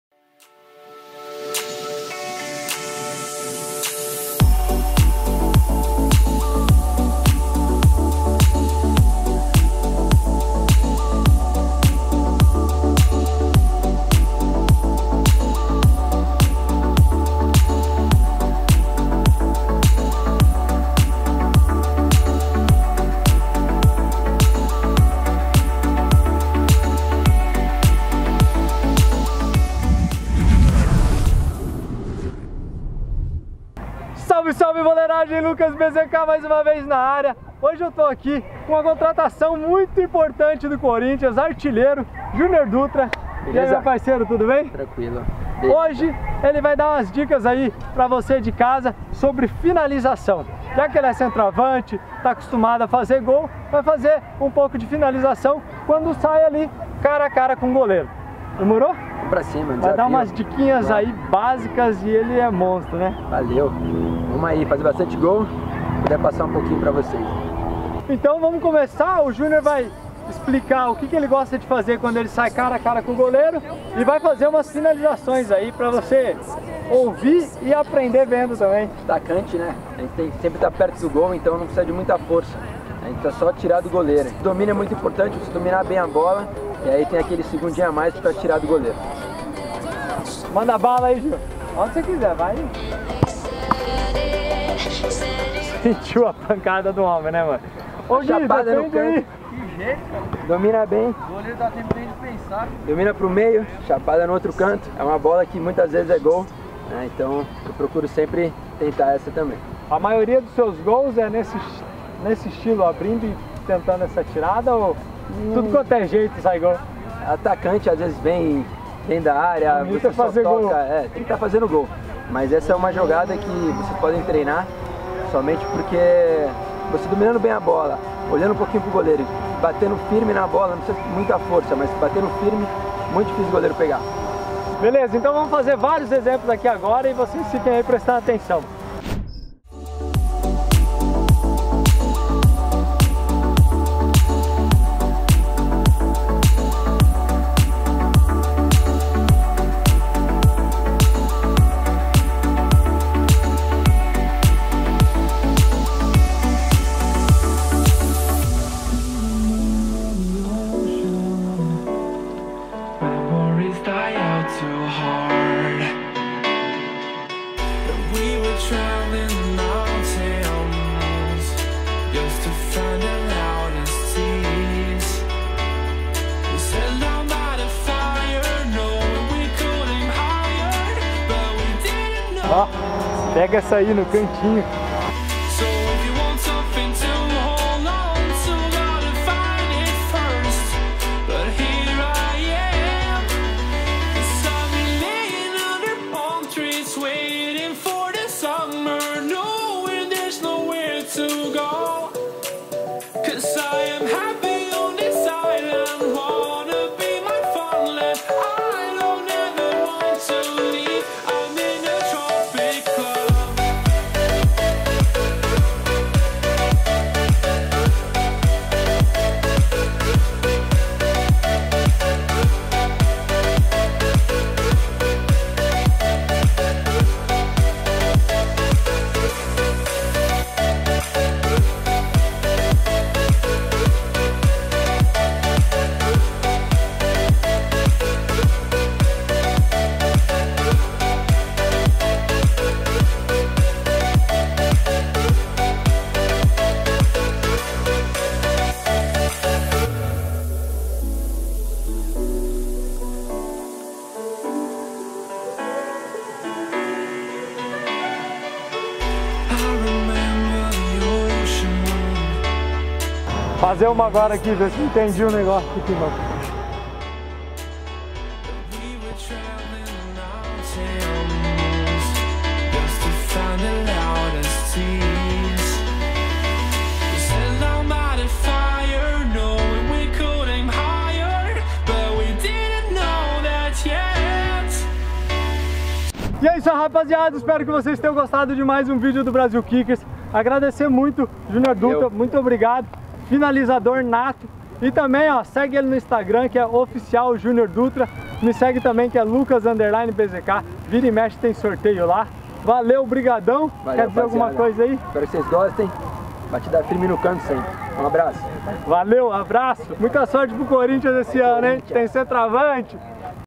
FINALIZAÇÃO COM JR. DUTRA DO CORINTHIANS! {BZK}
Finalização perto do gol não é força é jeito! (Jr. Dutra)
APRENDA FINALIZAÇÃO COM JR. DUTRA CORINTHIANS! {BZK}